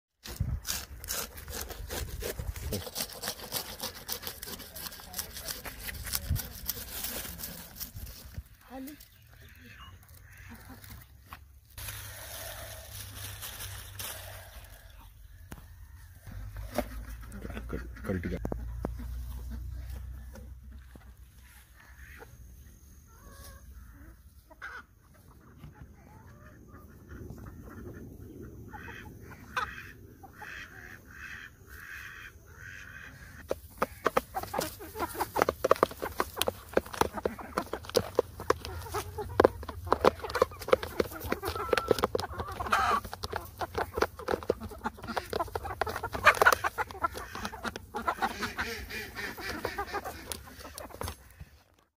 How to make maggots for Chicken | Convert Fish waste into Maggots
Making Maggots for your farm or backyard chicken is very easy. Just collect fish waste and put in a bucket for few hours and you will get live worms. Do not forget to make some holes in the basket to enter flies.

Baby chicks coming out of mom arm

Colour baby chicks

Mama and newly hatched chicks